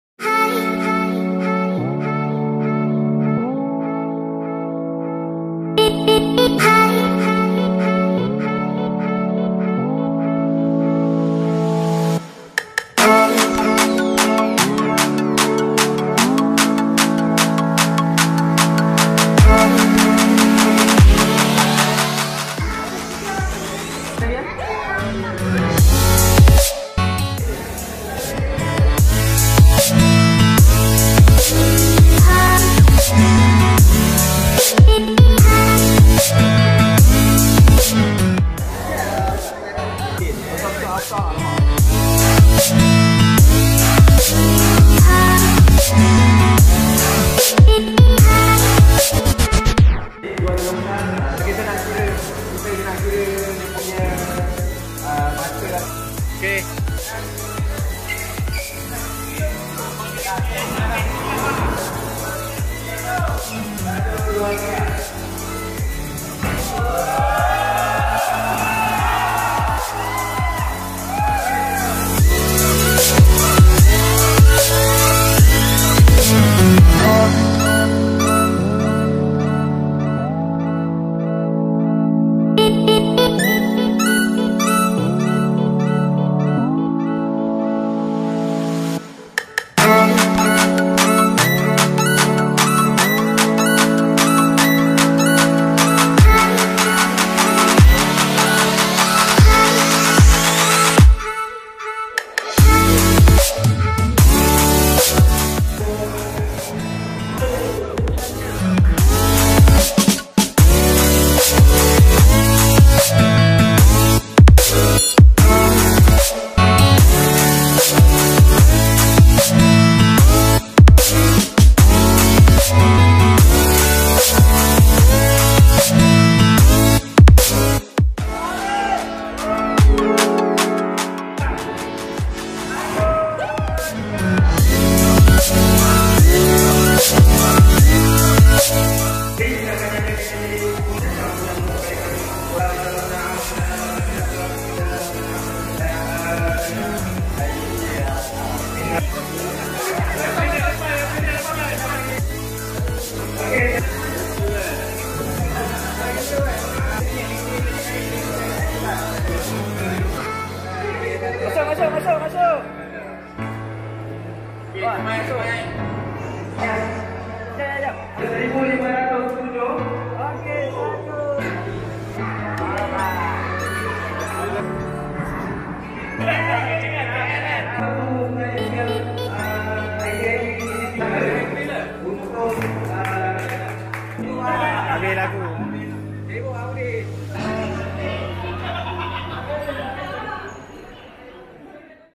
Honda BRV Owners Club ( HBOC) Bowling Tournement Mesamall Nilai
Tournement bowling HBOC berlangsung di The Bowlers Mesamall Nilai pada 4 Ogos 2019 anjuran HBOC N9.